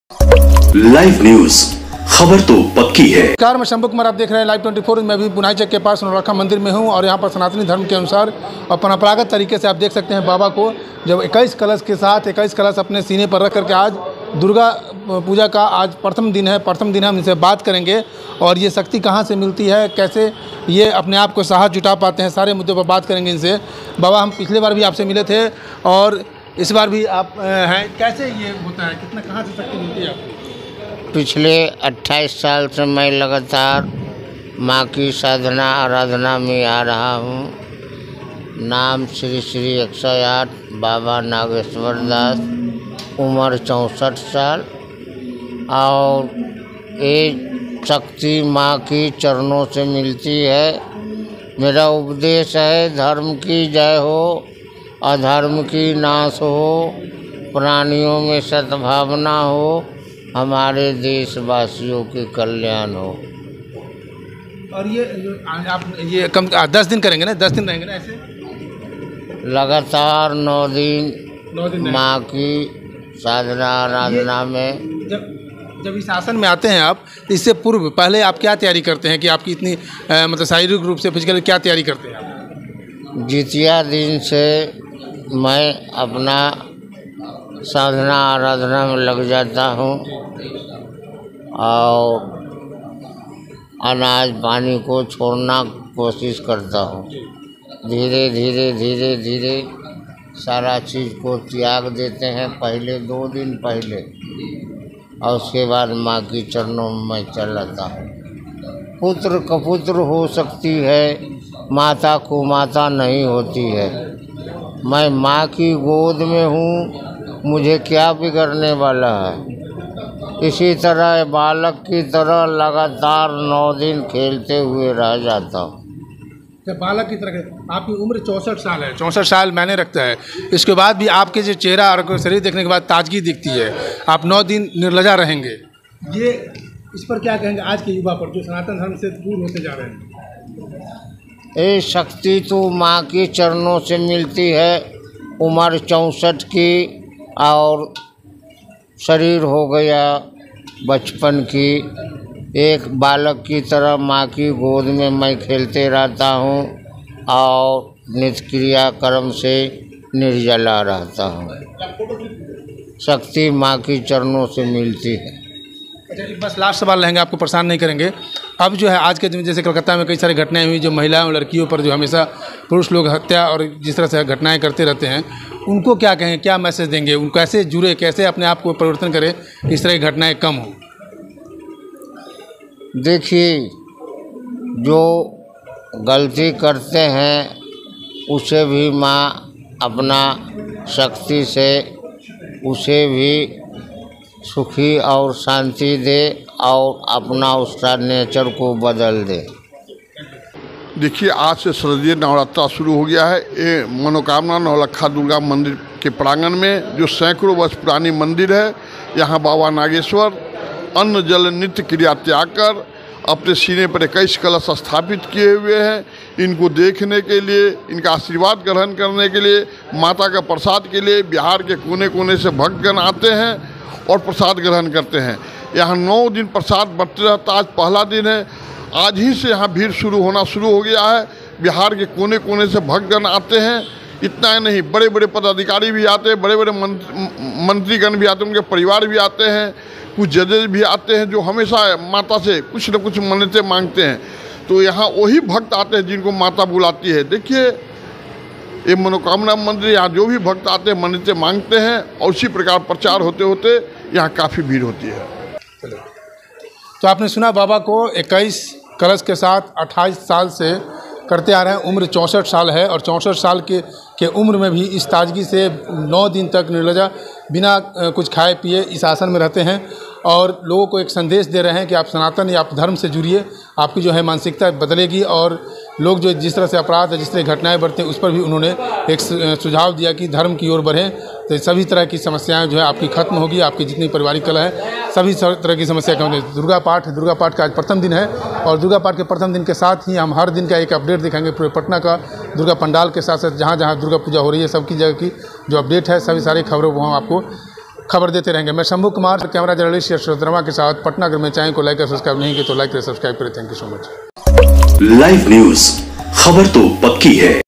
64 साल के बाबा पर मां दुर्गा का आशिर्वाद 28 साल से 21 कलश अपने सीने पर रख 9 दिन तक करते है उपवास
64 साल के बाबा पर मां दुर्गा का आशिर्वाद 28 साल से 21 कलश अपने सीने पर रख 9 दिन तक करते है उपवास

Live 24 News
Shambhu kumar